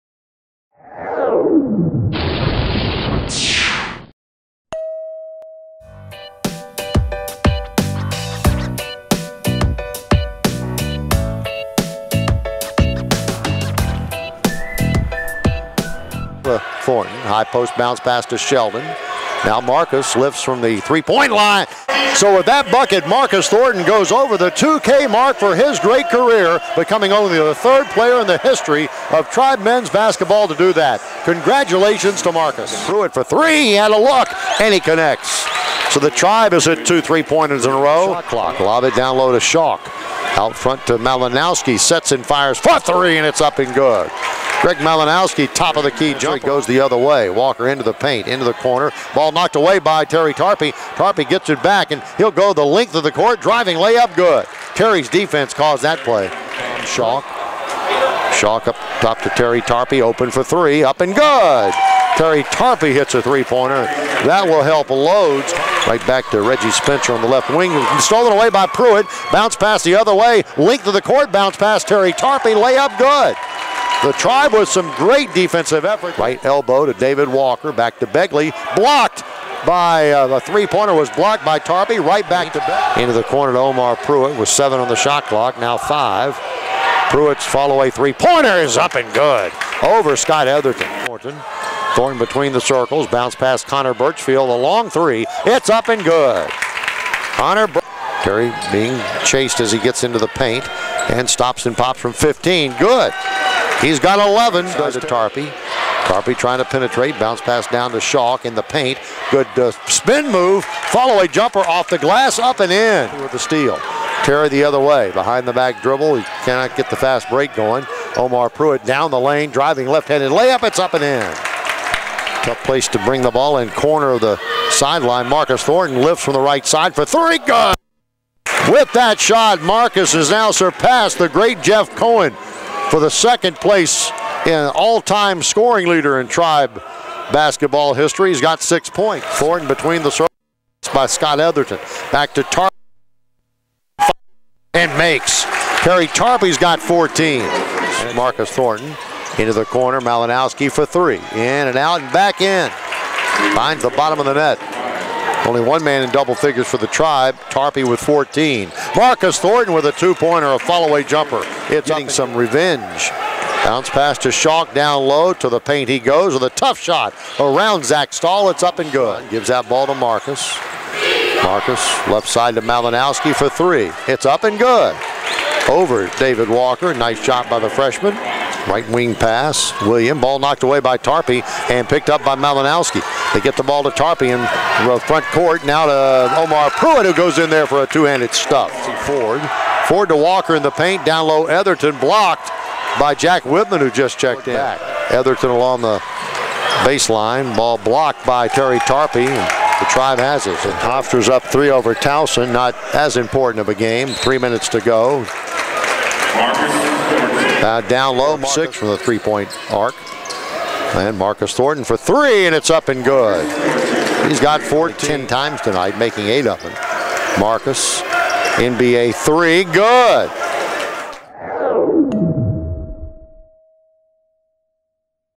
2014-15 William and Mary Men's Basketball Highlights at Northeastern
Check out the highlights from Tribe men's basketball's 75-64 setback at Northeastern on Feb. 18.